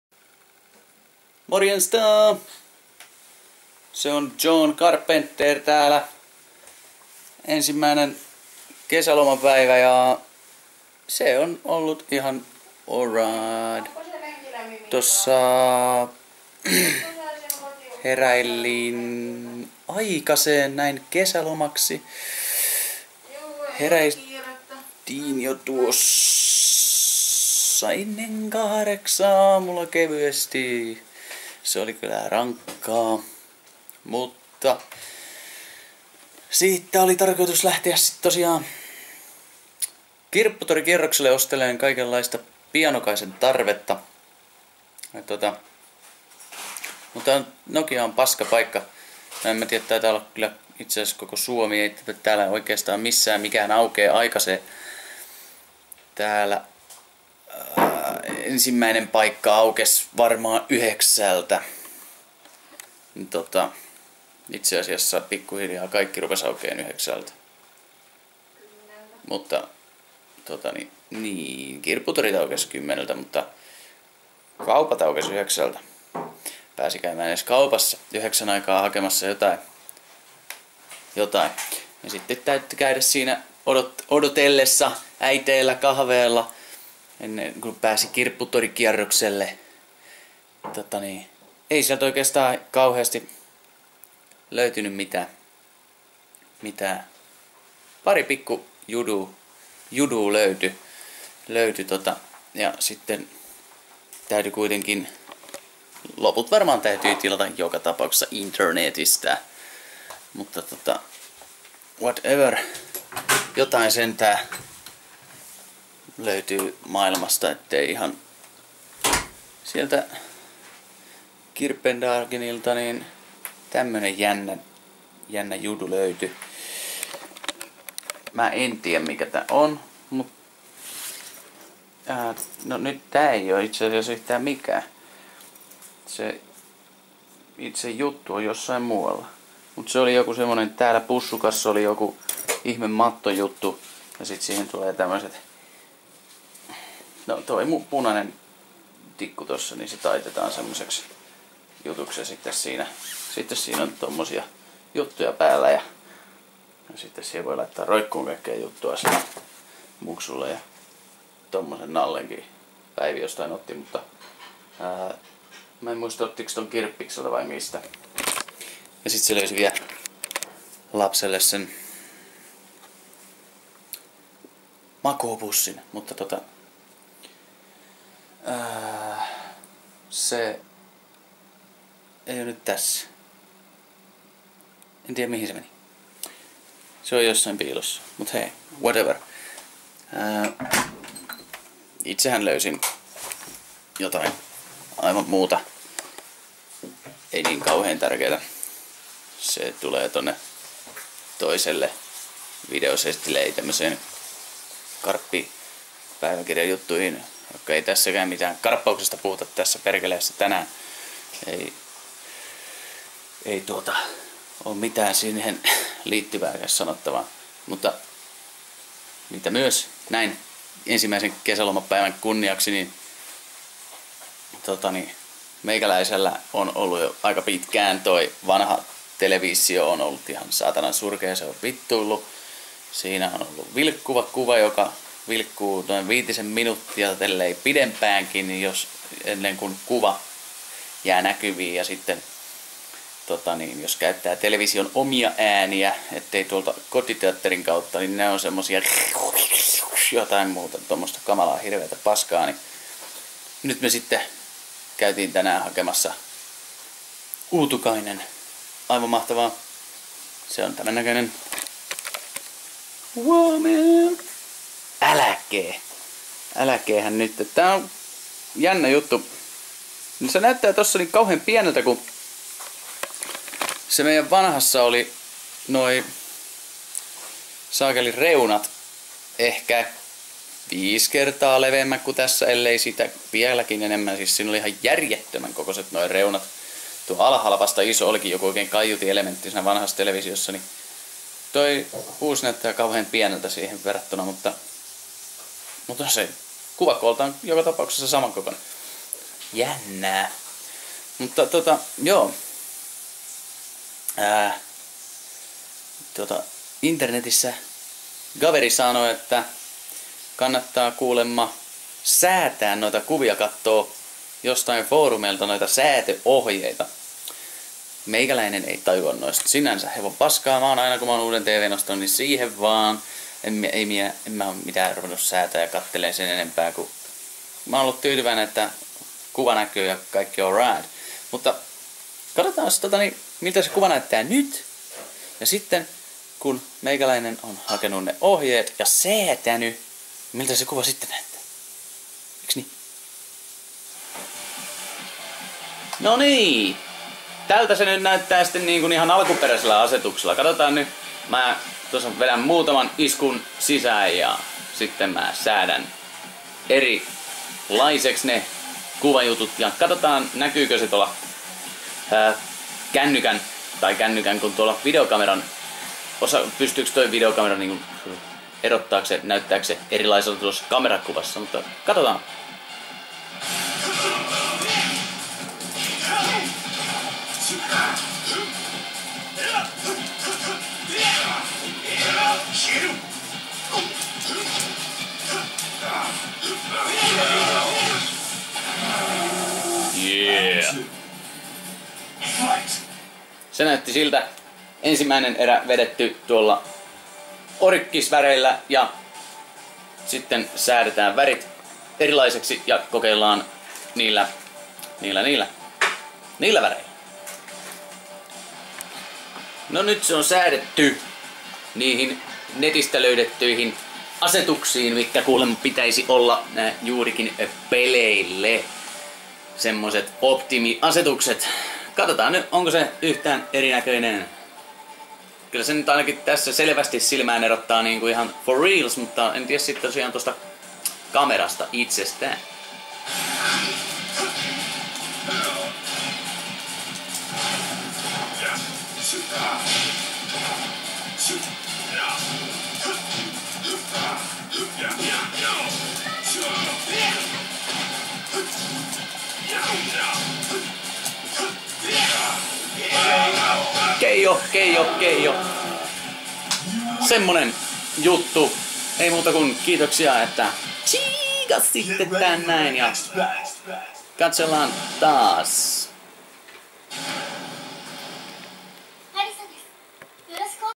Karpin päiväkirja Vol.18
Ensimmäisen lomapäivän tunnelmia ei oikeestaan mitään karppaukseen viittaavaakaan tällä kertaa mutta ei kai aina tarvitsekkaan :)